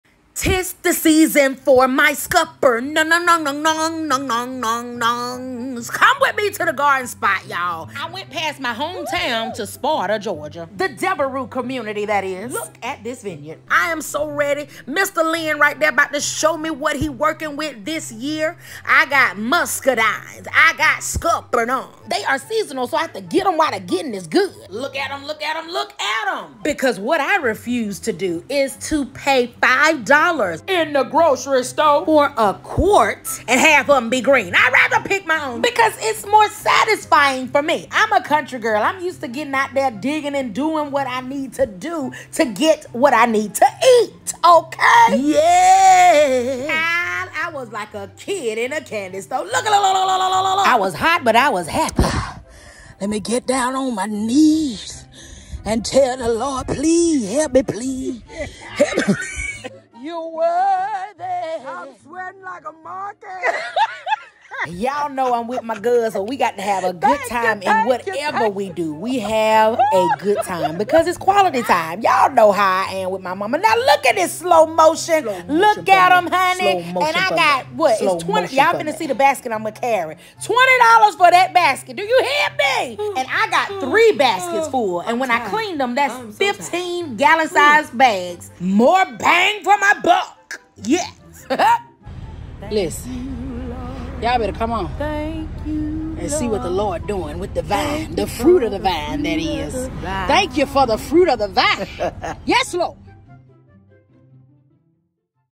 Picking Scuppernongs and Muscadines
🗣Y’all! I crave scuppernongs all year! They are seasonal which only makes me appreciate them more!

They are made from the harvest of the grapes but better… to me. The muscadines are the dark purple ones. 😋

I have been picking them with my mom since my teenage years! Here is what our latest trip was like to “The Garden Spot” in Sparta, Georgia! grapes fruit of the vine